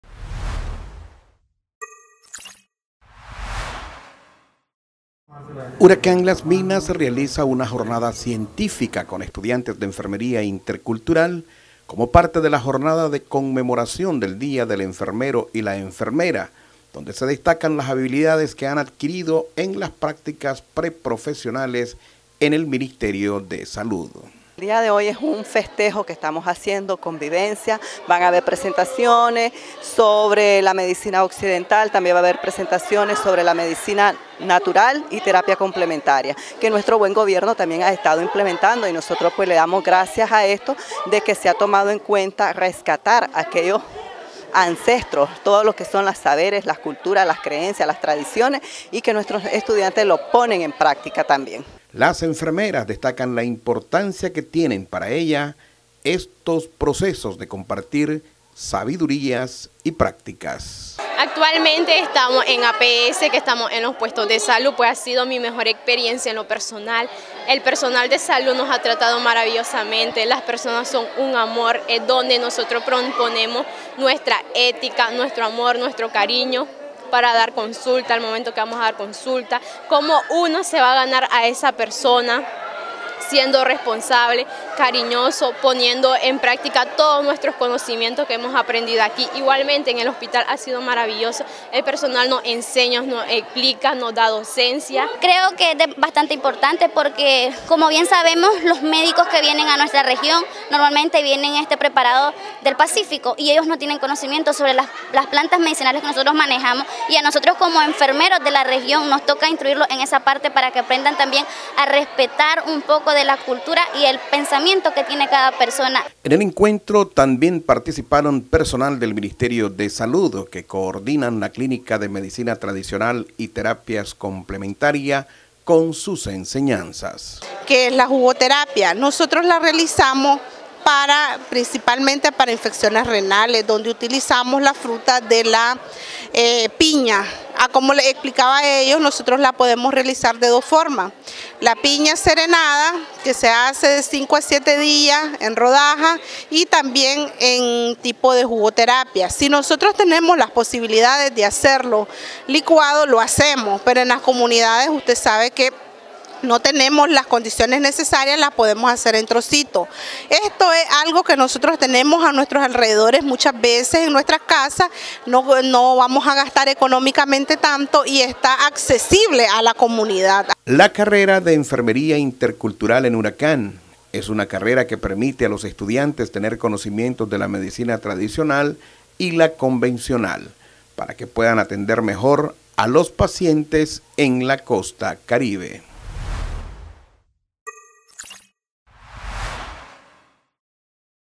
𝗜𝗻𝘁𝗲𝗿𝗰𝗮𝗺𝗯𝗶𝗼 𝗜 117 estudiantes de enfermería intercultural abordaron experiencias vividas en Hospital
117 estudiantes de la carrera de enfermería intercultural de URACCAN Recinto Las Minas, realizaron una jornada de intercambio donde abordaron las experiencias que han vivido durante las prácticas realizadas en el Hospital Carlos Centeno de Siuna, pero, además en las campañas de vacunación impulsadas por el Gobierno de Nicaragua en el Caribe Norte.